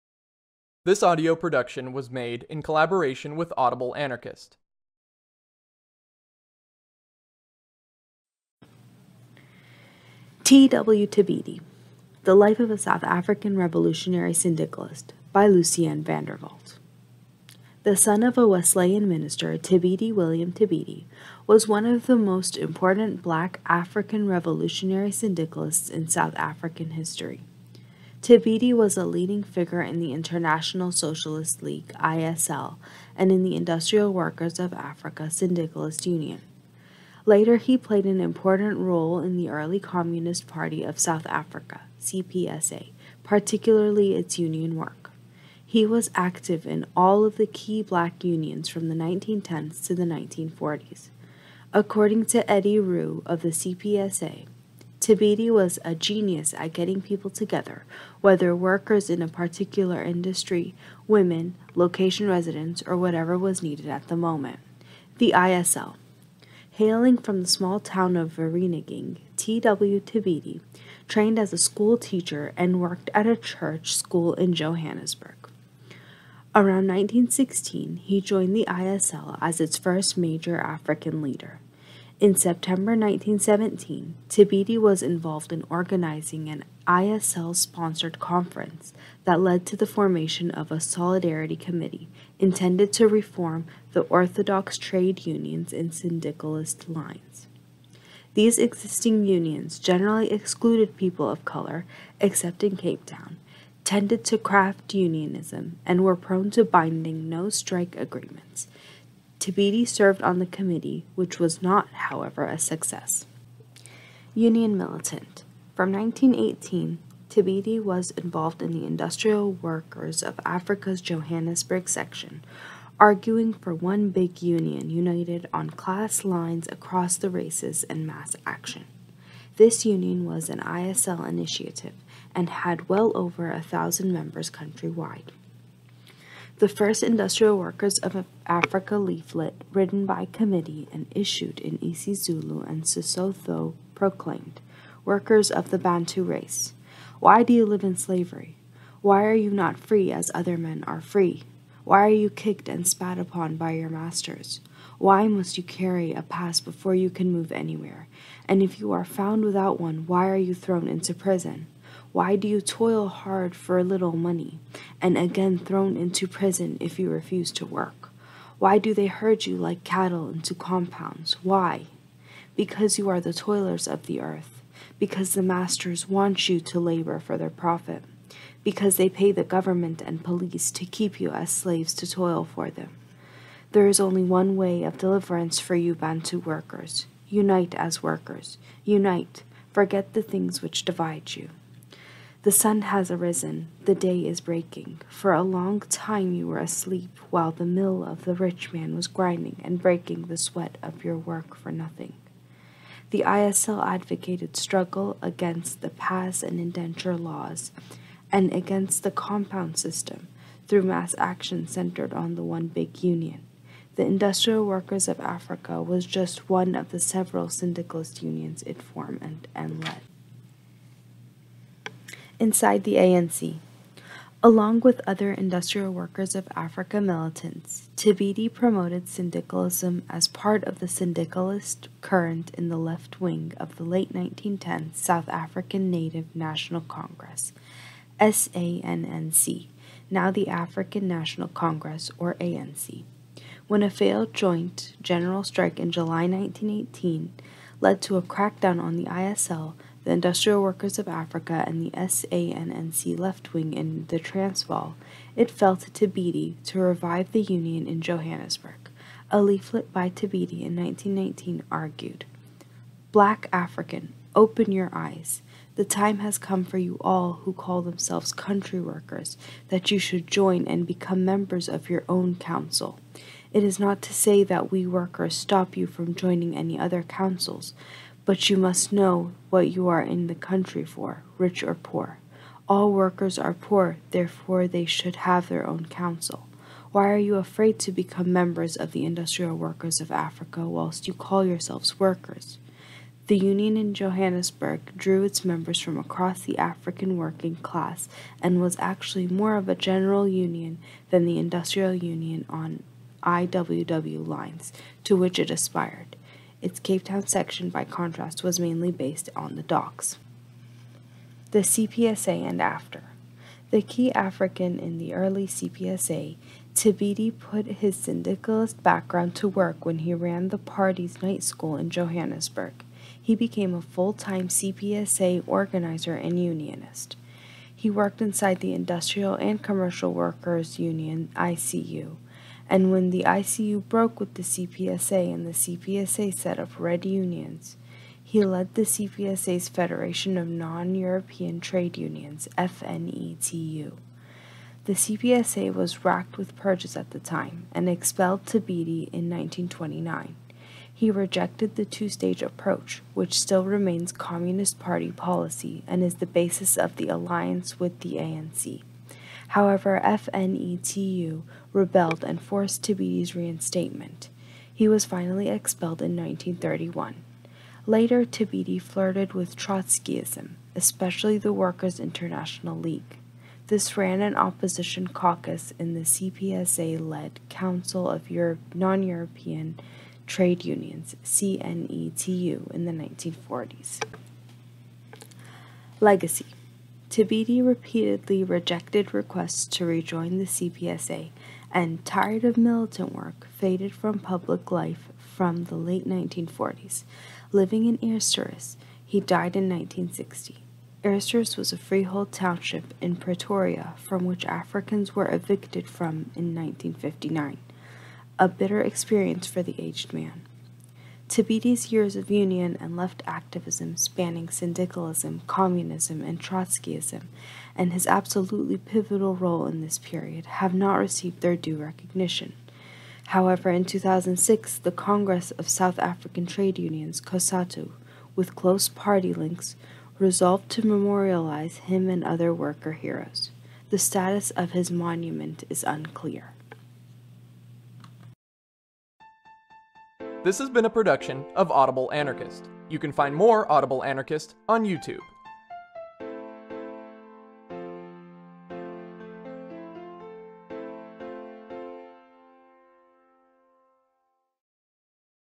TW Thibedi The Life of a South African Revolutionary Syndicalist
A short history of the TW Thibedi a South African Syndicalist.